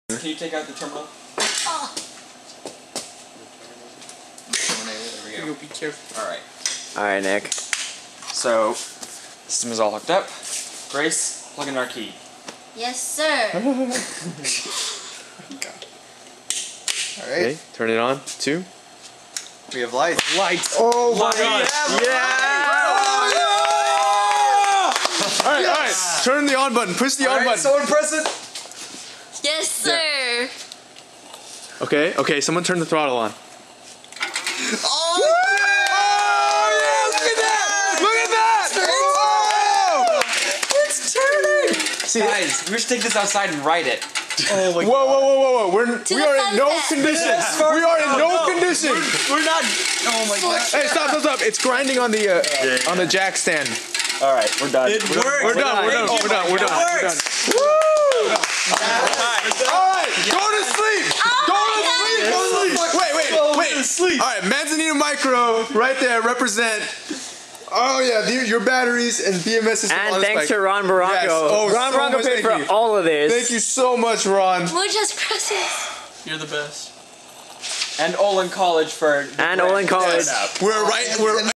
REVO: e-Bike First Success
The first successful test of REVO's first project, the e-Bike, at 6:30AM. Check out the links below to learn more about REVO.